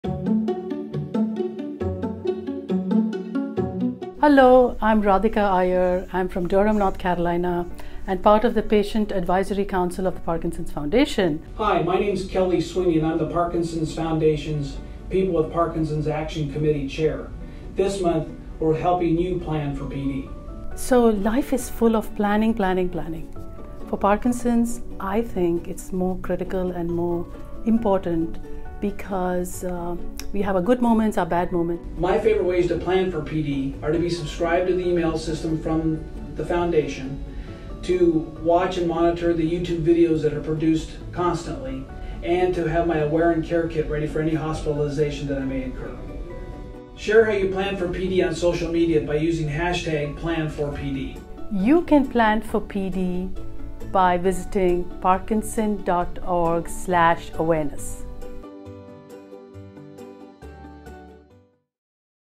Parkinson's Awareness Month 2020: Let's
Download your free Parkinson's Awareness Month calendar for ideas on how to Plan4PD throughout April

If this last month has taught us anything, it is how important it can be to have thoughtful plans in place. While we cannot always anticipate things beyond our control, we can better manage life’s fluctuations through
preparation and community support.

Every day with Parkinson’s disease can feel different from days prior and the ones that lie ahead. Having a plan (or many adjustable plans) in place can better prepare us for the challenges Parkinson’s may present.

This month, we want to encourage people to take actionable steps to Plan4PD and share ways that help improve their everyday lives with Parkinson's.